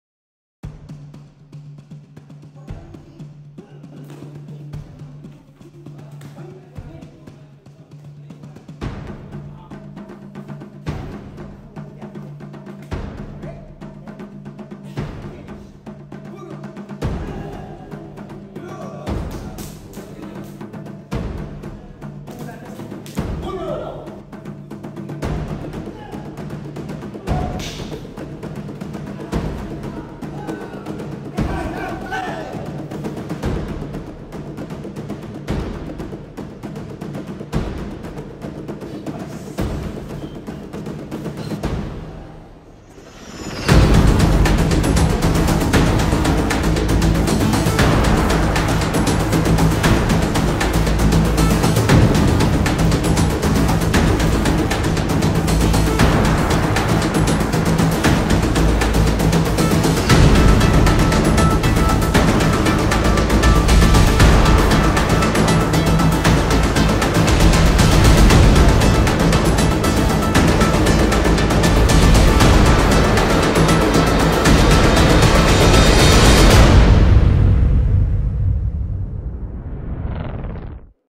Self defense- knife fighting system. Paano ang pagdepensa sa kutsilyo? Tara na!
BTPMA Knife fighting training and self-defense.